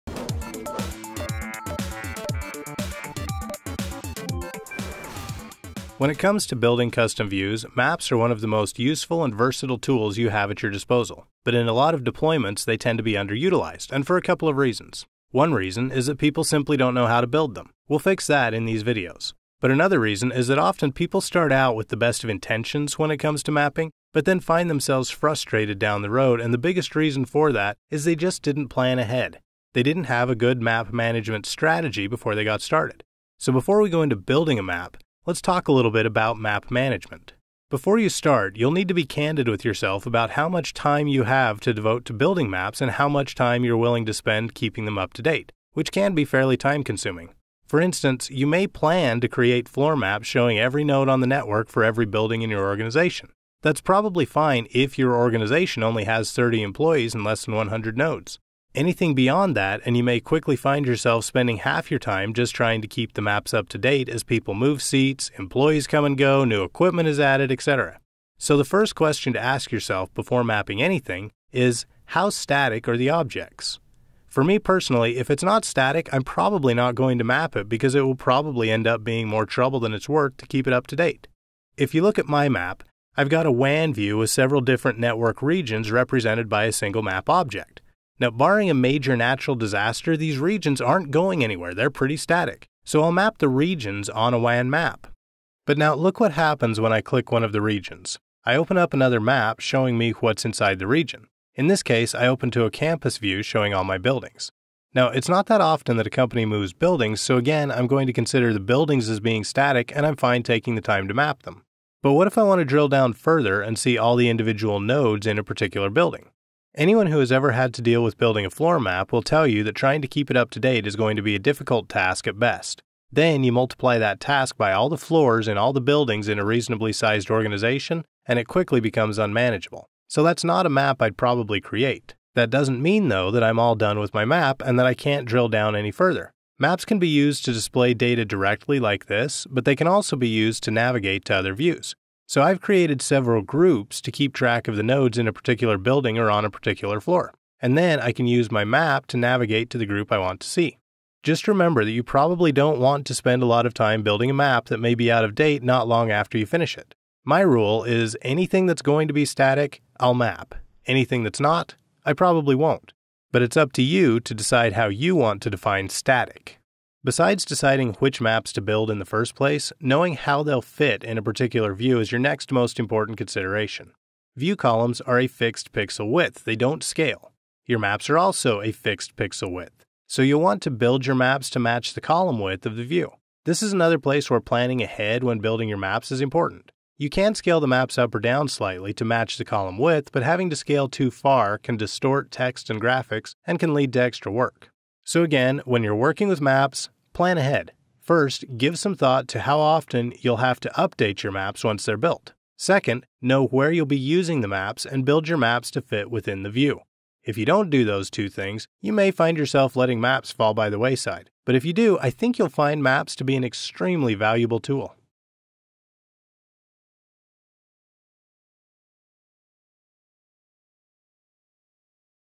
SolarWinds Network Performance Monitor Training: Planning and Managing Your Maps
When it comes to building custom views, maps are one of the most useful and versatile tools you have at your disposal. This video provides best practices of what to consider before you start building your maps.

• Map only static objects. If objects move, you need to adjust their location on maps, and it is difficult to keep maps up-to-date.
• Build maps to match the column width of your Orion Web Console views. Rescaling maps in views results in distorting of icons and texts.